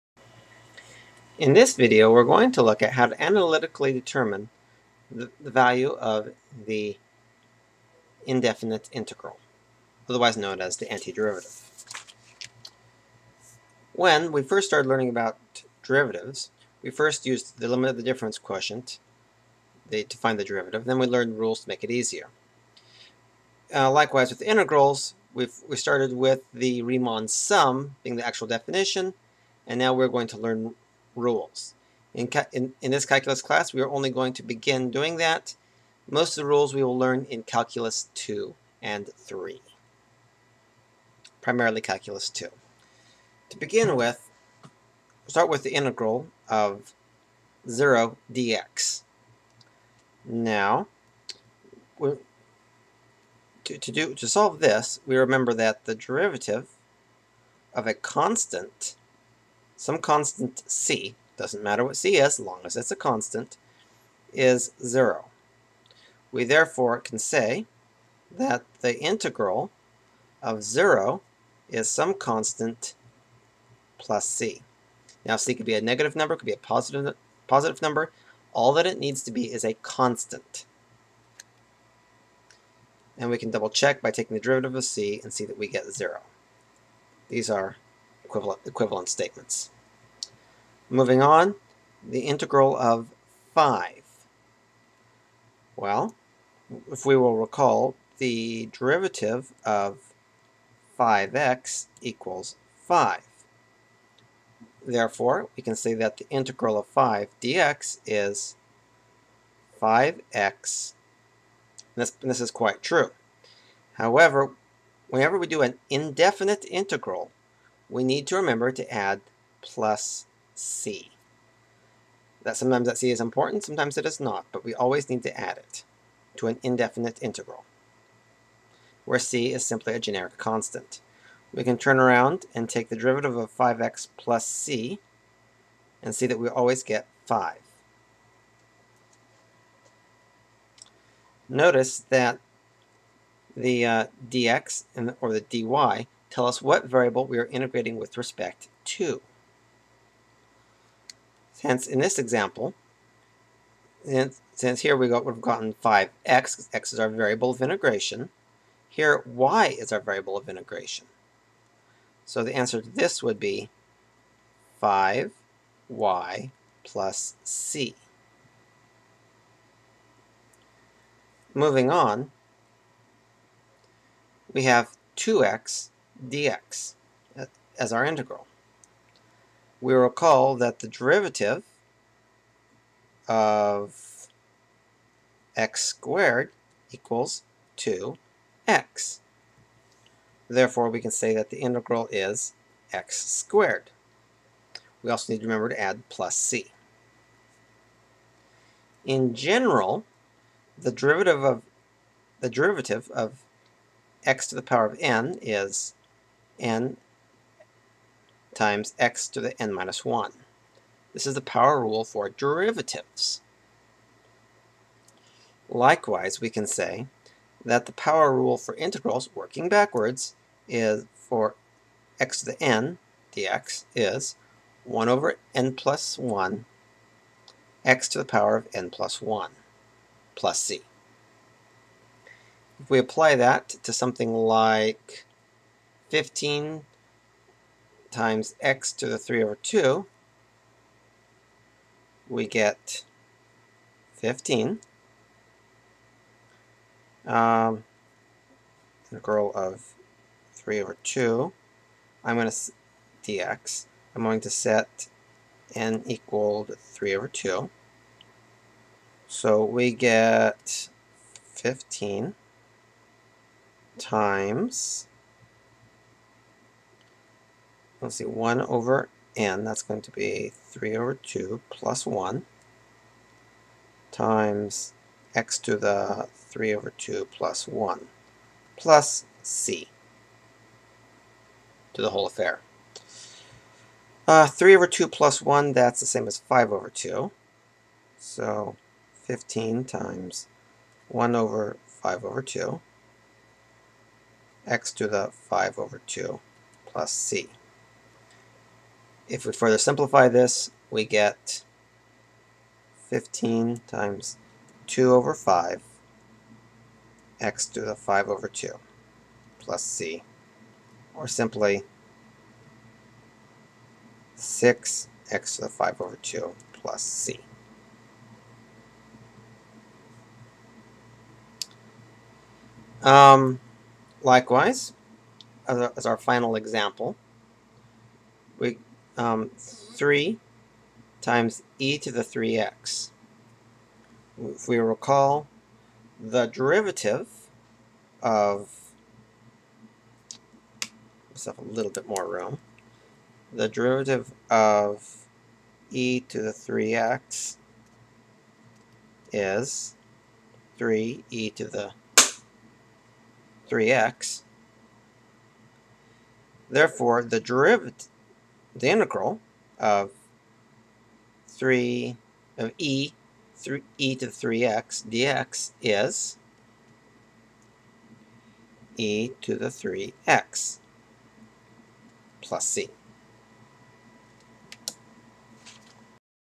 Determining the Indefinite Integral
How to solve an indefinite integral - Ottawa University Calculus 1 Week 8.